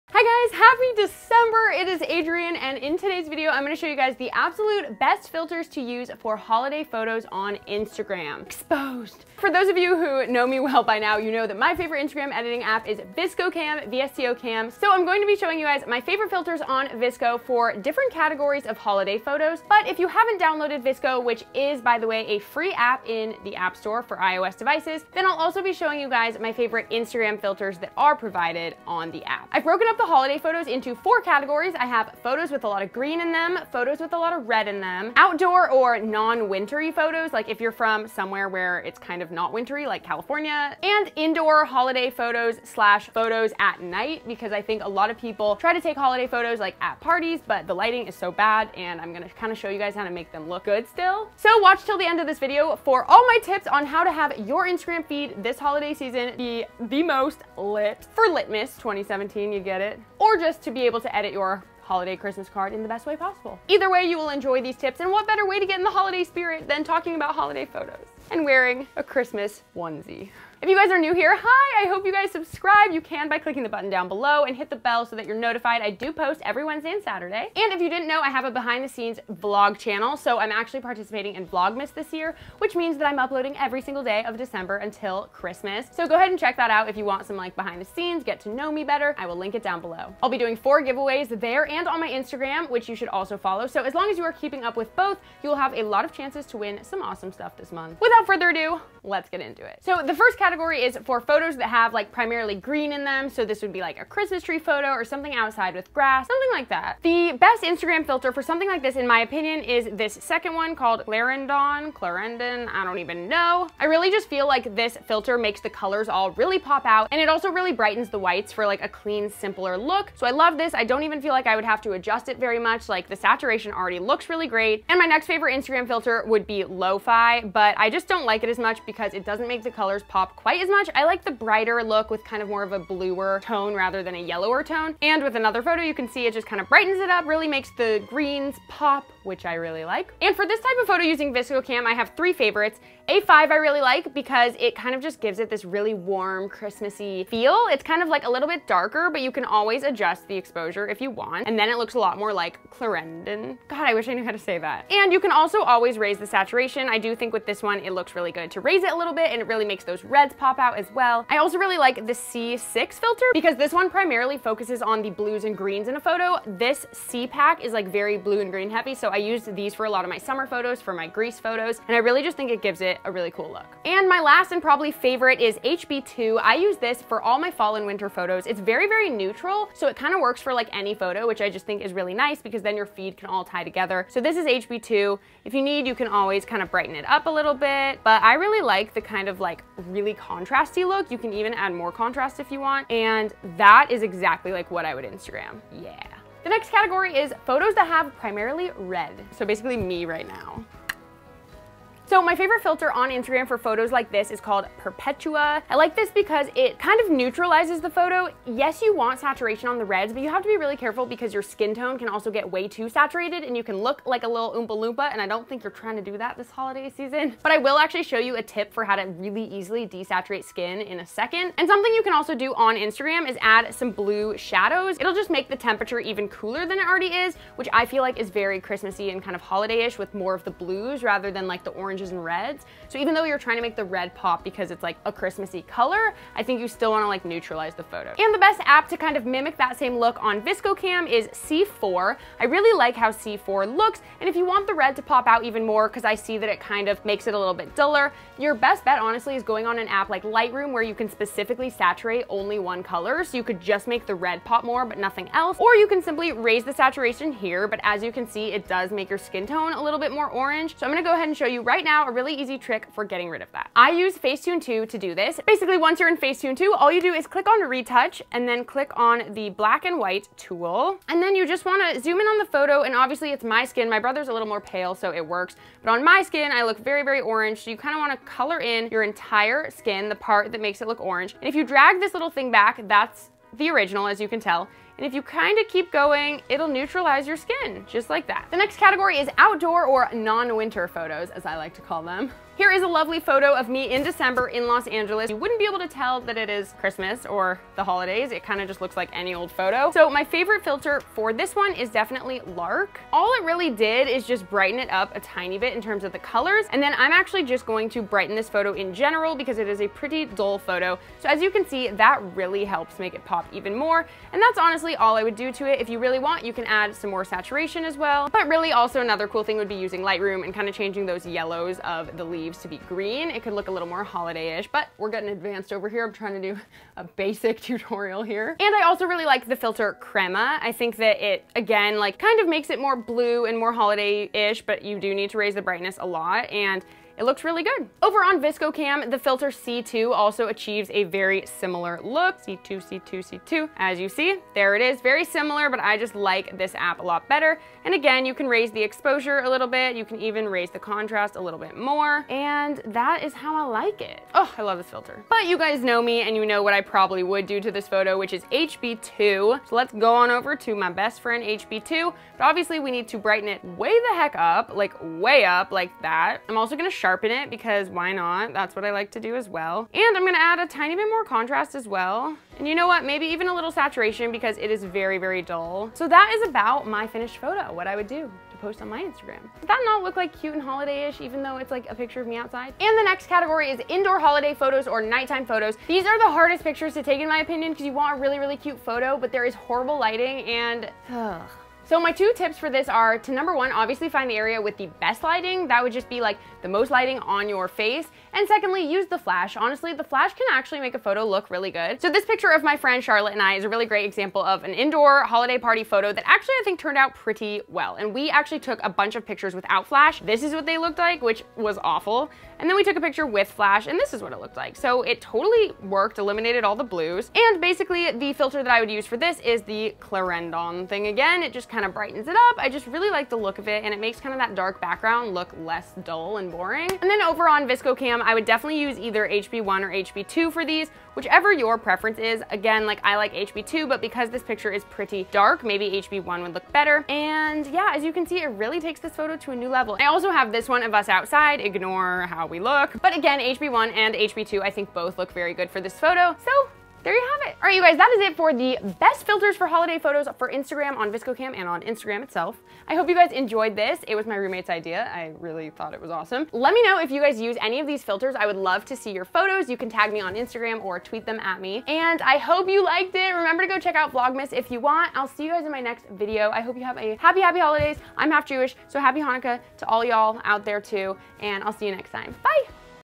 BEST INSTAGRAM FILTERS FOR CHRISTMAS PHOTOS REVEALED!
WORLD'S BEST FILTERS to use on Holiday Photos for Instagram!
TOP VSCO & Instagram FILTERS!! Achieve the perfect holiday Instagram theme!!

Today, I'm showing you guys the BEST filters to use in each of these categories of holiday photos:

-Photos with RED primarily
-Photos with GREEN primarily
-Outdoor or "non-holiday" photos
-Indoor / Holiday Party photos

→ FILTERS
PHOTOS WITH GREEN IN THEM
IG Filter: Clarendon, Lo-Fi
VSCO: A5, C6, HB2

PHOTOS WITH RED IN THEM
IG Filter: Perpetua
VSCO - C4

OUTDOOR NON-WINTER PHOTOS
IG Filter: Lark +brightness
IG Filter: Crema +brightness
VSCO: C2
VSCO: HB2 +exposure, +saturation, +sharpen

INDOOR HOLIDAY PARTY PHOTOS (or night time photos)
-Find area with best lighting
-Use flash!
IG Filter: Clarendon
VSCO: HB1, HB2

→ LET'S BE FRIENDS because I looove guys! ←

→ TRAVEL LINKS! ←